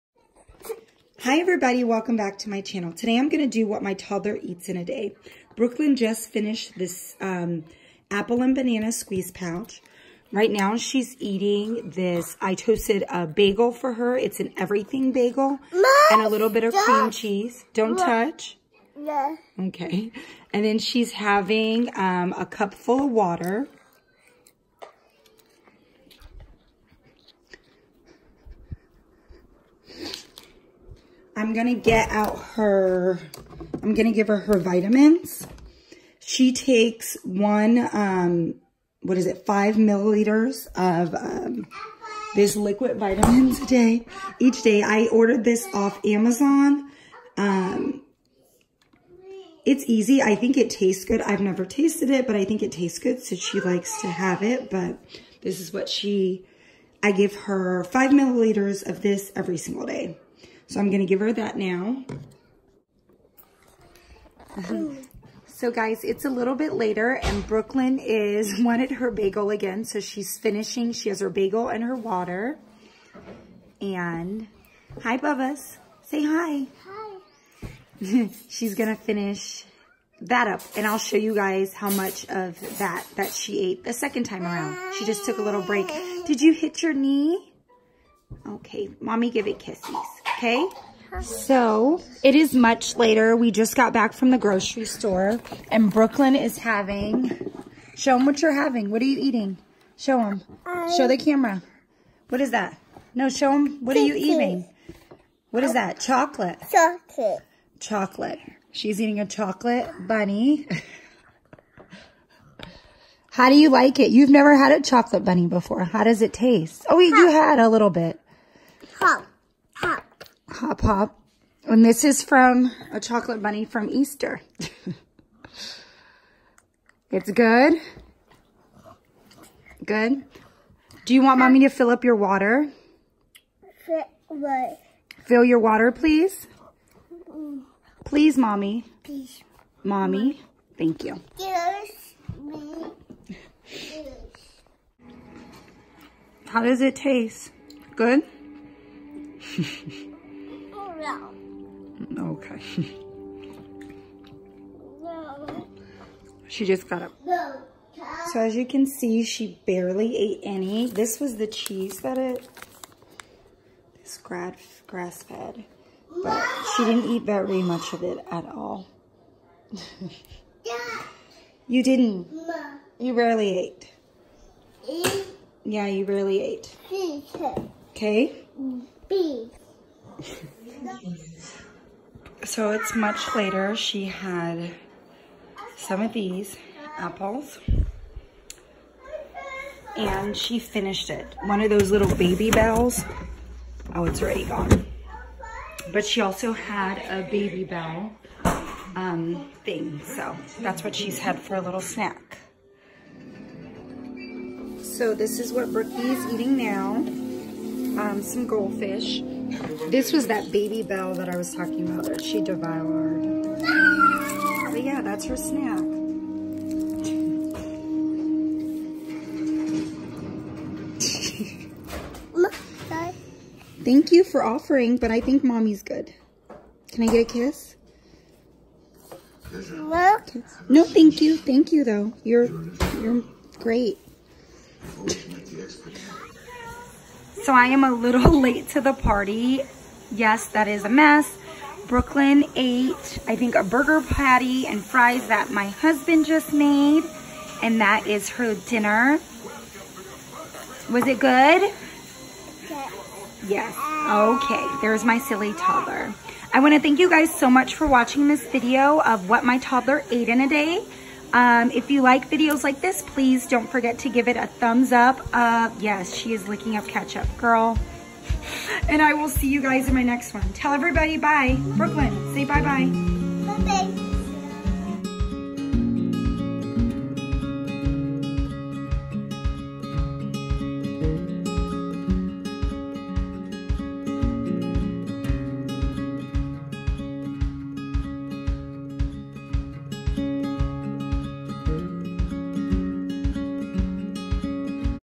What My Toddler Really Eats In A Day 2019
I hope you guys enjoy this real life what my toddler eats in a day. Love Dream